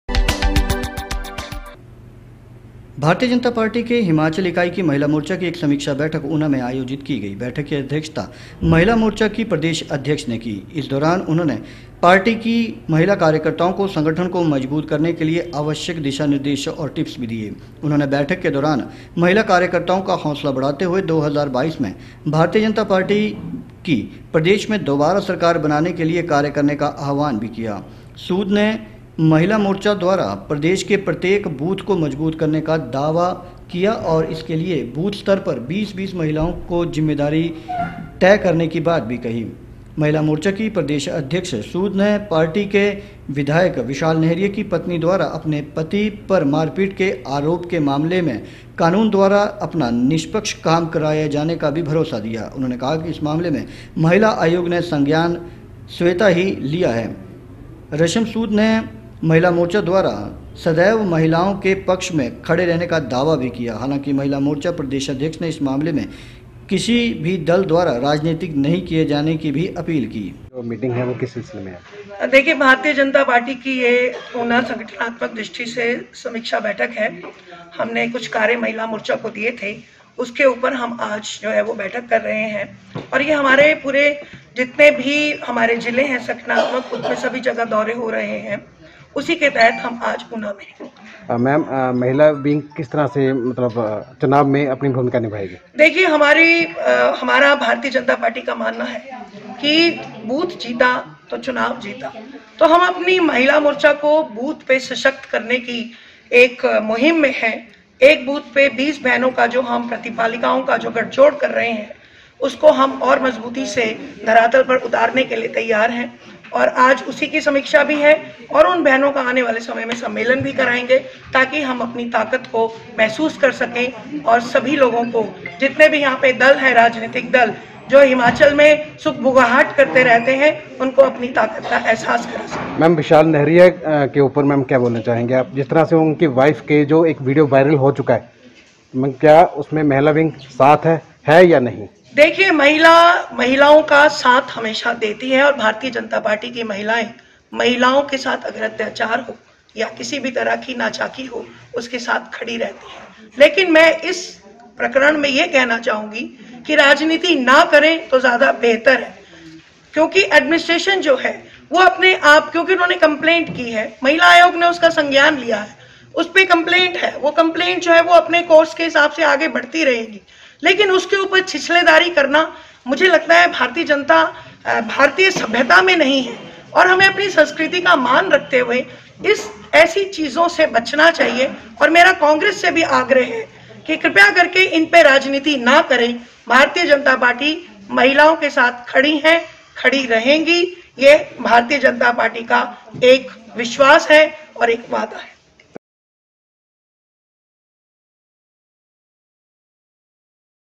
UNA MAHILA MORCHA बीजेपी महिला मोर्चा ऊना में हुई समीक्षा बैठक
भारतीय जनता पार्टी की हिमाचल इकाई की महिला मोर्चा एक समीक्षा बैठक आज उना में हुई । बैठक की अध्यक्षता महिला मोर्चा की प्रदेश अध्यक्ष रश्मि सूद ने की । इस दौरान उन्होंने पार्टी की महिला कार्यकर्ताओं को संगठन मजबूत करने के लिए आवश्यक दिशा निर्देश और टिप्स दिए । उन्होंने बैठक के दौरान महिला कार्यकर्ताओं का हौसला बढ़ाते हुए 2022 में भारतीय जनता पार्टी की प्रदेश में दोबारा सरकार बनाने के लिए कार्य किए जाने का आह्वान किया । रश्मि सूद ने महिला मोर्चा द्वारा प्रदेश के प्रत्येक बूथ को मजबूत बनाने का दावा किया और इसके लिए बूथ स्तर पर 20-20 महिलाओं की जिम्मेदारी तय किए जाने की बात कही । महिला मोर्चा की प्रदेश अध्यक्ष रश्मि सूद ने पार्टी के विधायक विशाल लहरिया की पत्नी द्वारा अपने विधायक पति पर मारपीट के आरोप के मामले में कानून द्वारा अपना निष्पक्ष कार्य किए जाने का भरोसा दिया ।उन्होंने इस मामले को महिला आयोग द्वारा भी संज्ञान में लिए जाने का दावा किया । रश्मि सूद ने महिला मोर्चा द्वारा सदैव महिलाओं के पक्ष में खड़े होने का भी दावा किया ।